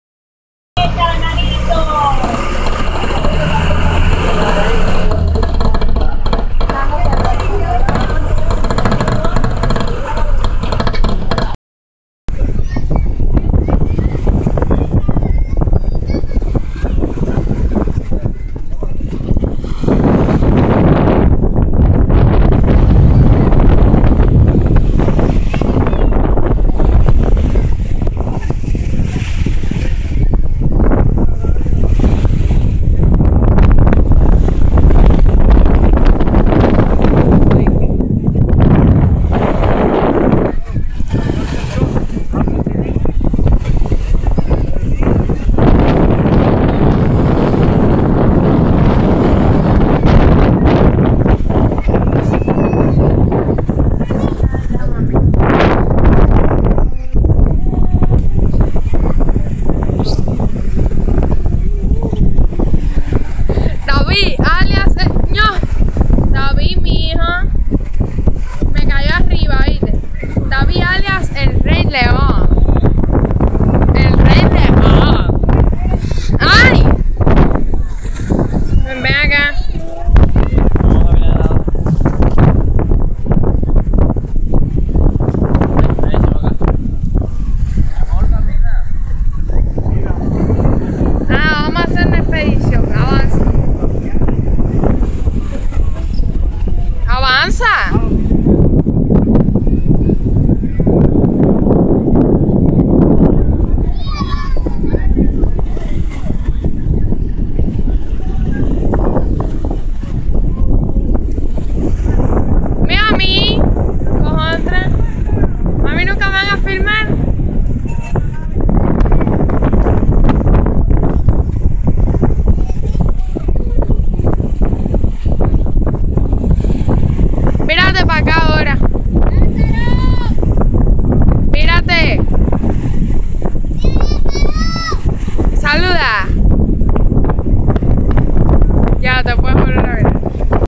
Nuevitas, la playa y ballenatos, Camaguey, Cuba
Mi sobrinita filmó esto cuando fueron a Nuevitas (nosotros somos de Camagüey ciudad) Así que para ustedes: Nuevitas la bella! Y, por supuesto, un saludo a todos los cubanos y en especial a los Nueviteros que viven fuera, quienes seguramente reconocerán su playita, que probablemente tanto extrañen!
Los Ballenatos han sido declarados área protegida, por los valores que atesoran, entre ellos, la Anolis Jubar Ballaenarum, una lagartija con cresta única en el mundo, muy vistosa y extraña, cuyo rabo semeja la hoja fina y alargada de una planta . También sobresalen sus poblaciones de iguanas e insectos y las aves acuáticas y migratorias que allí anidan.
Por estar considerada como una zona de alta fragilidad ecológica y con una rica biodiversidad, dada por sus características geológicas y los procesos eólicos y costeros que se observan y por la belleza natural que los acompaña, son estas pequeñas islas el mayor orgullo de los nueviteros. Se les conoce como Ballenato del Medio –el de mayor altura, con 54 metros sobre el nivel del mar–, Grande y Chiquito.